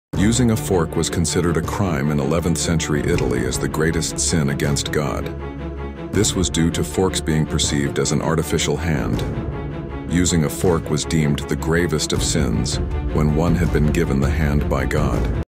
Fork Usage in 11th Century Italy: Sin Against God - Historical Scandal and Traditional Violation!
Explore! A historical scandal from the 11th century in Italy where using a fork was considered a major sin against God. This video narrates the extraordinary story and delves into the details of the traditional violation. Embark on a journey through history and culture, unveiling the curtain on a memorable era! 🕰️🇮🇹